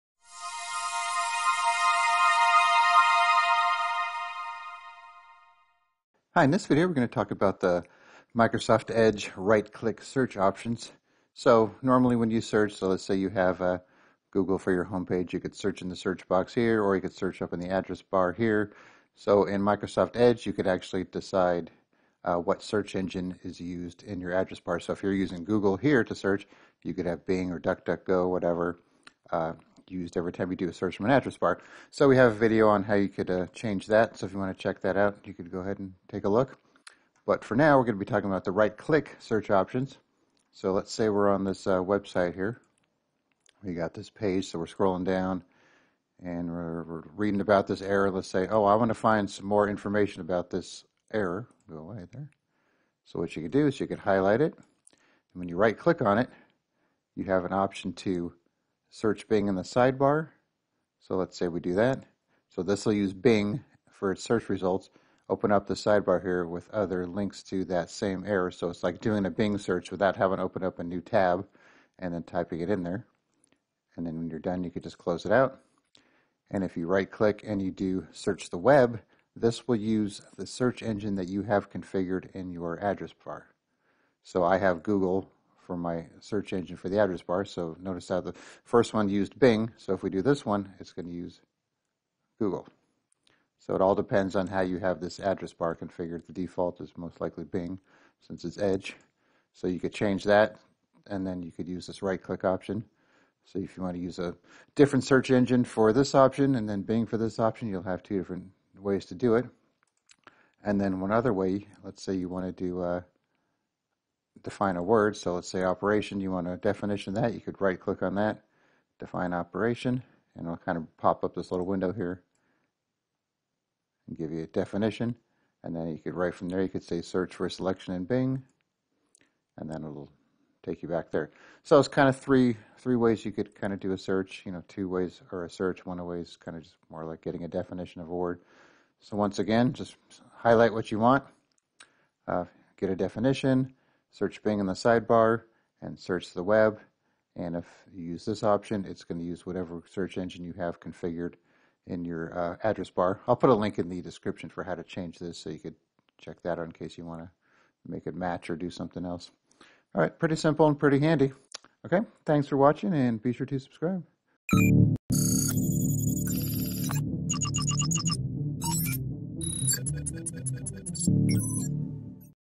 Microsoft Edge Right Click Search Options
If you use Microsoft Edge as your web browser, you can right click on any webpage to get a variety of options and tools. There are a few options in this right click menu that can help your find what you are looking for and they are Search the web, Search in the Sidebar and Define. If you highlight a word or phrase and then use one of these tools, it can help you find what you are looking for.

Here is the video for how to change the search engine used in your address bar.